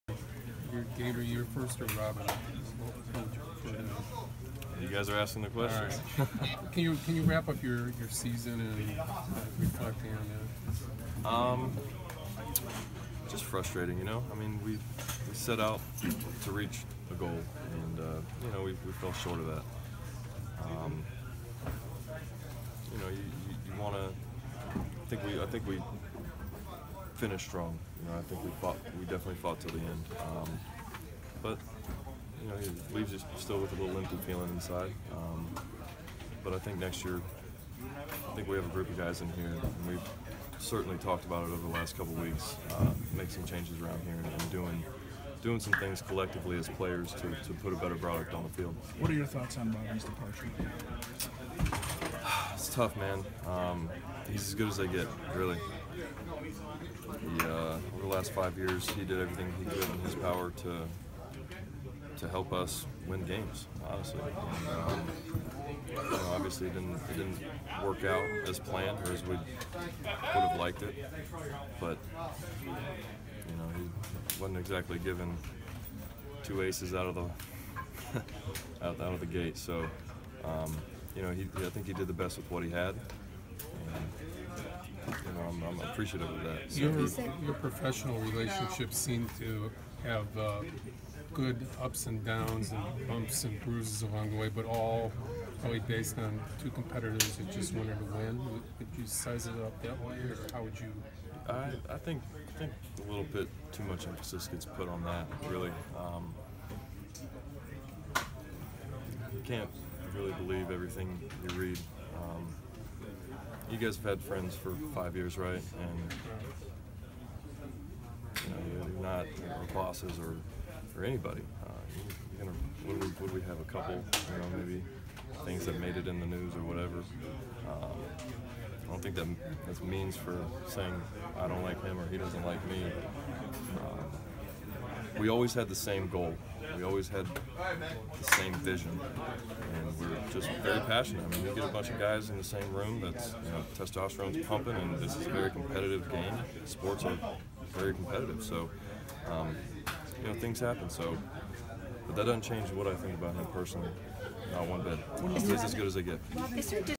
Chris Sale
After last game of 2016, end of Robin Ventura era